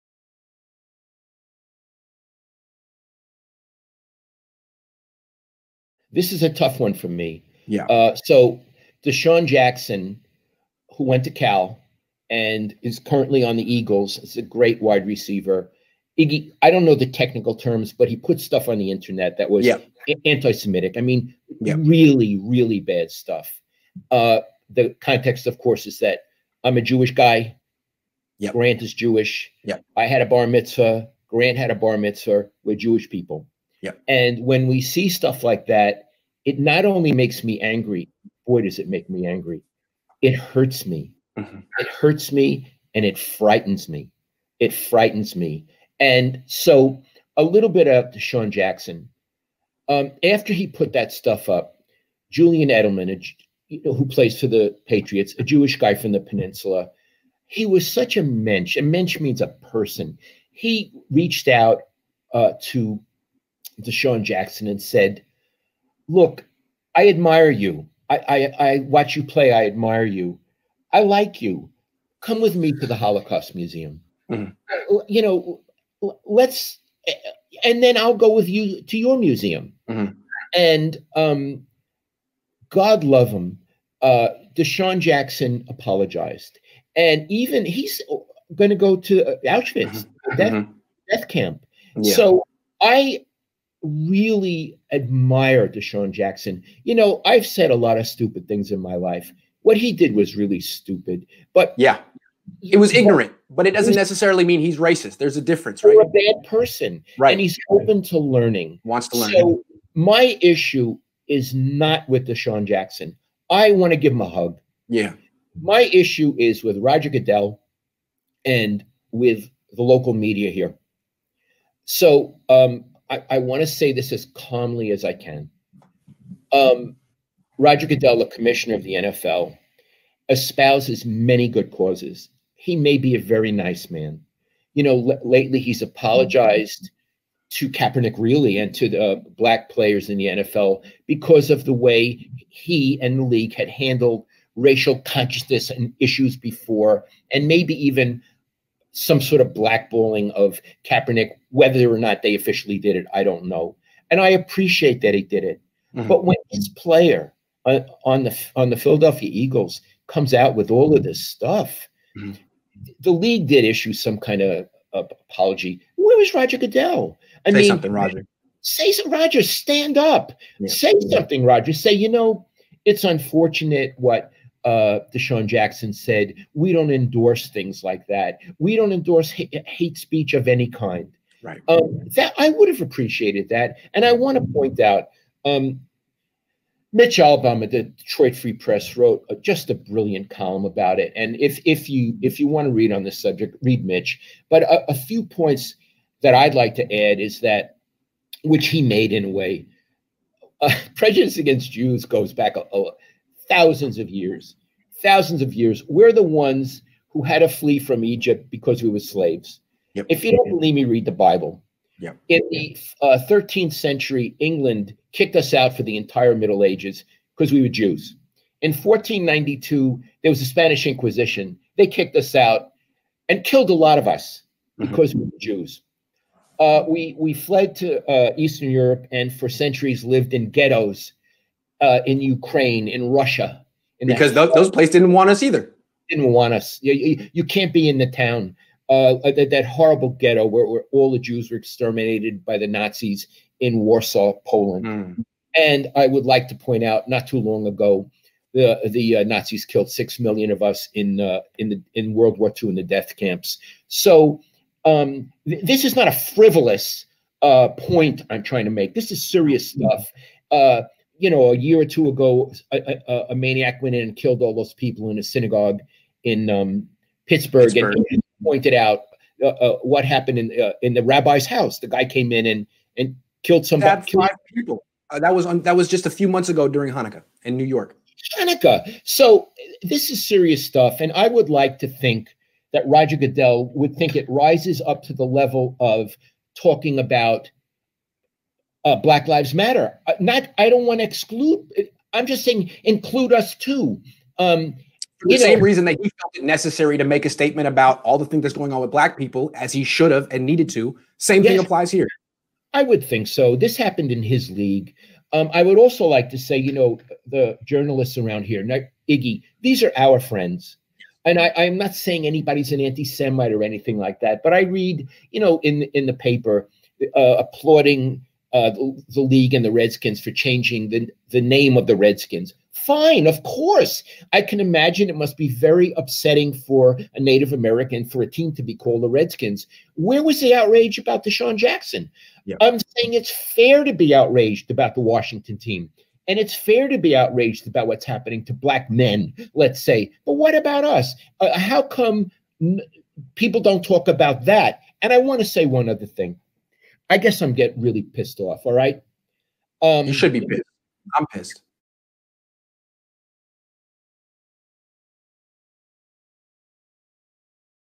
Roger Goodell and the Bay Area Media need to Say Something about DeSean Jackson's Antisemitic Posts
On this week's episode of The Cohn Zohn, Lowell explains why Roger Goodell and the Bay Area media need to break their silence about Philadelphia Eagles wide receivers DeSean Jackson's antisemitic posts.